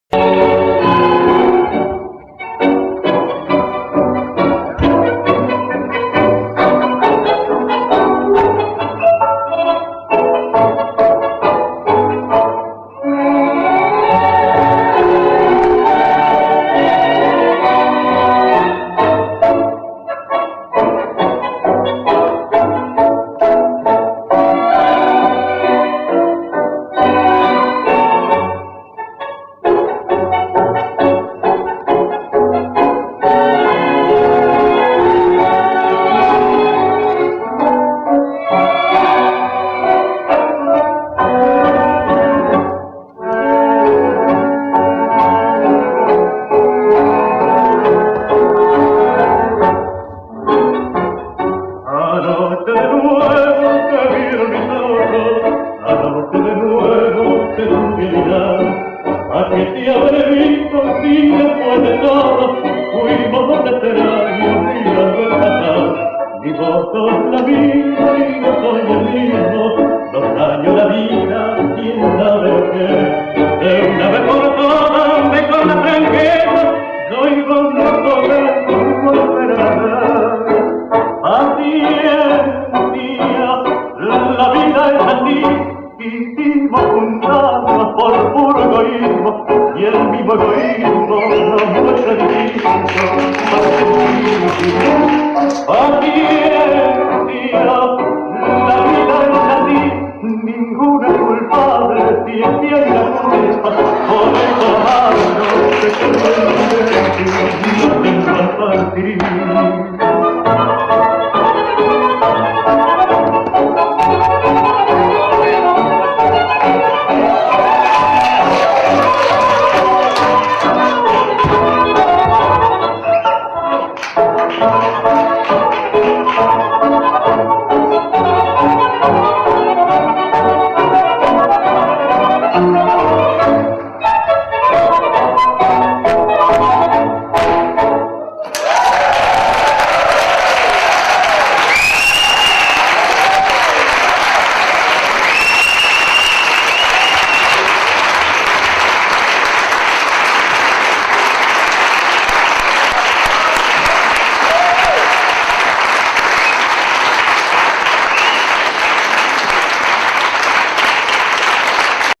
Gustavo Rosas y Gisela Natoli beim 5. Bonner Tangofestival
5. Bonner Tangofestival Mai 2011 mit Gustavo Rosas & Gisela Natoli, Juanma Nieto & Rachel Greenberg, Harald Rotter & Susanne Illini, Otros Aires, Cuesta Arriba, Duo Ranas, Porque Tango.